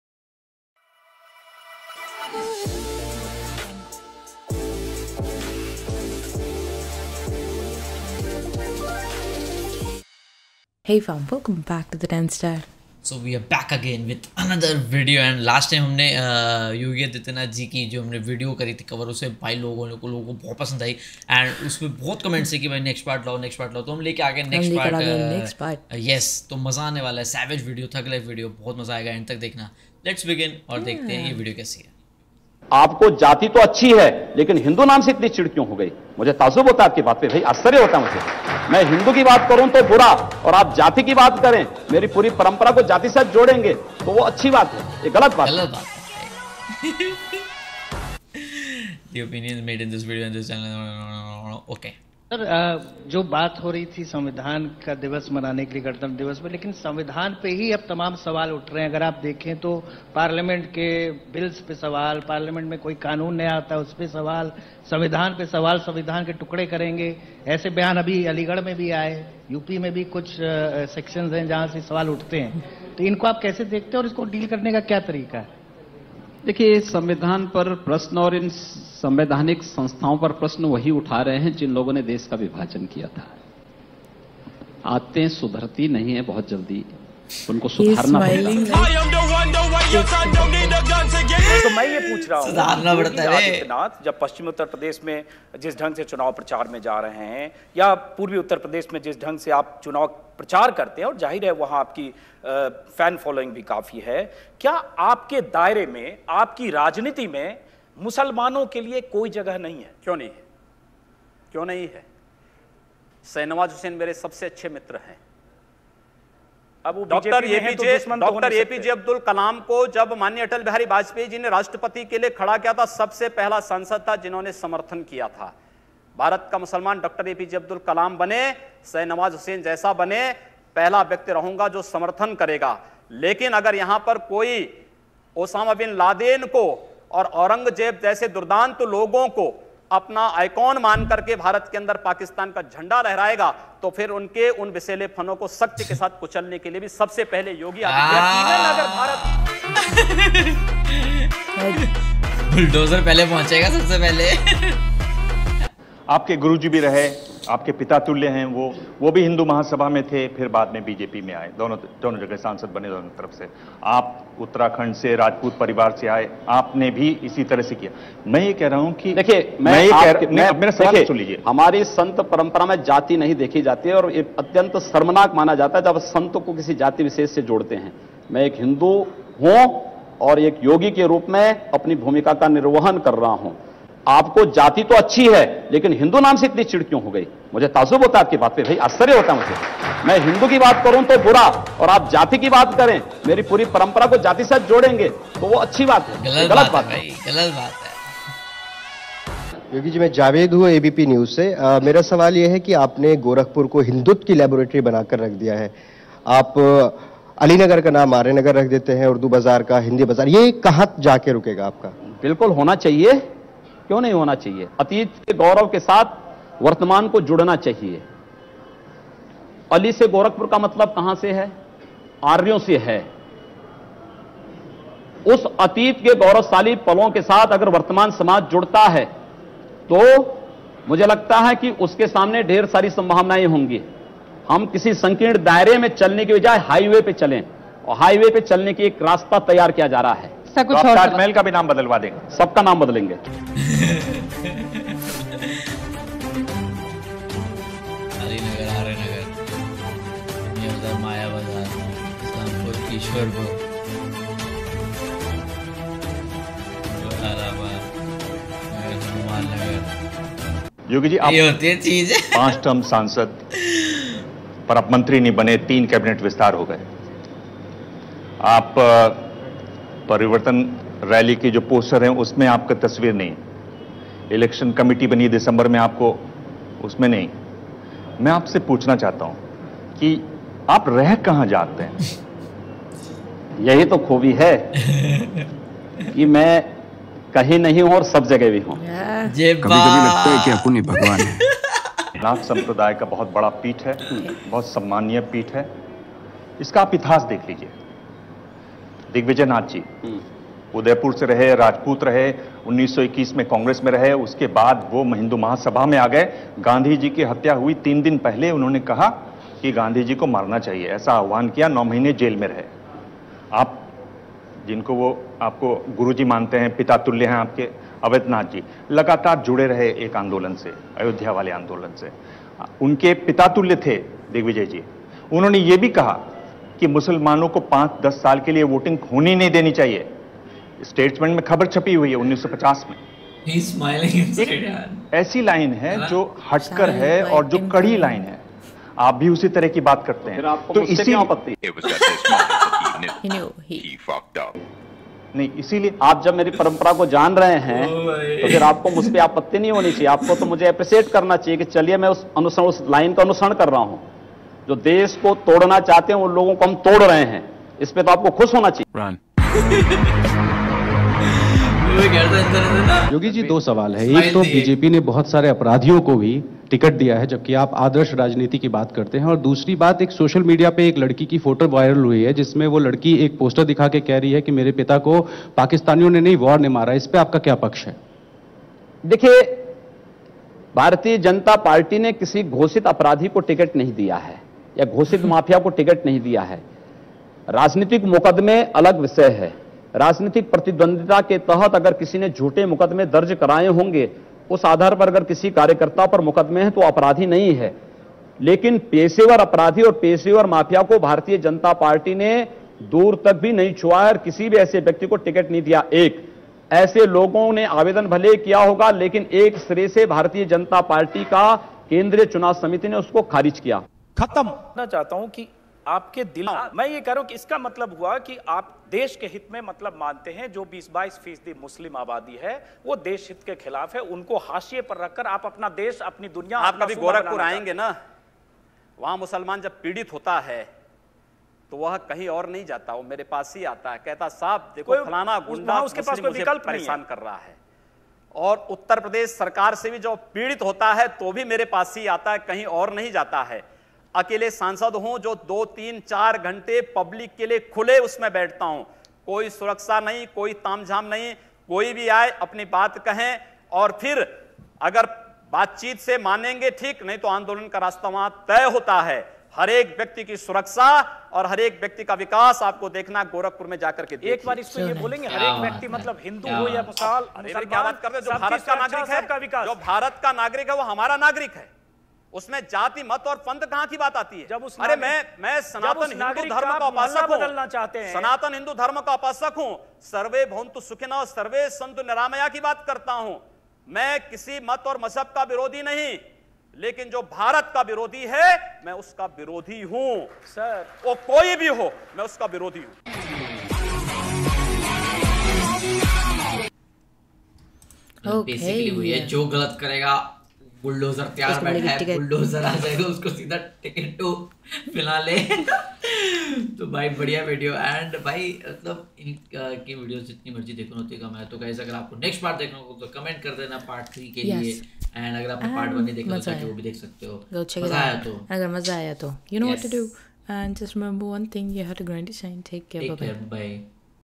CM Yogi Adityanath Thug Life #2 😈| Yogi Adityanath Attitude Videos Reaction 🔥
In this video we gave reaction to Yogi Adityanath Thug Life 😈| Yogi Adityanath Attitude Videos🔥